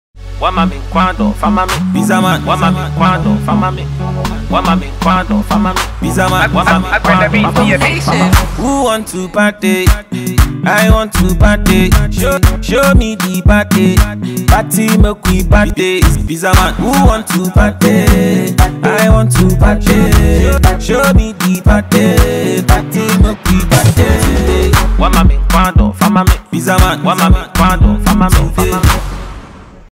DR LIKEE AT PIZZAMAN CHICKENMAN SUAME BRANCH
How Dr. Likee and his crew enjoyed meals from the new branch of PIZZAMAN CHICKENMAN inside Maakro, KUMASI.